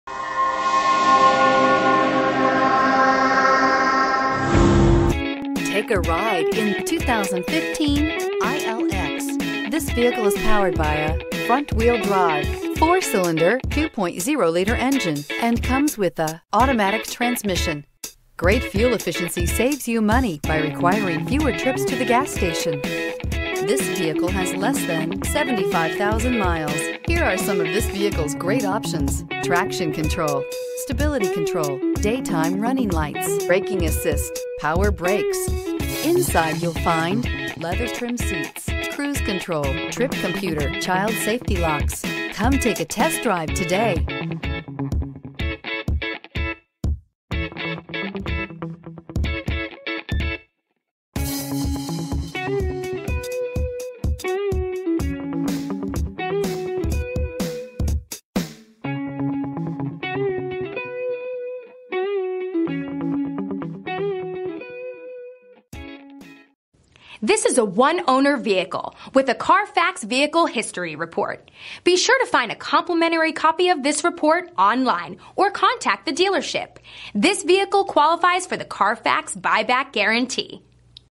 2015 Acura ILX Columbus GA FE001833
2015 Acura ILX 2.0L

Rivertown Ford
1680 Whittlesey Road

Pick up this Silver 2015 Acura ILX, available today at Rivertown Ford. This could be the one you've been searching for.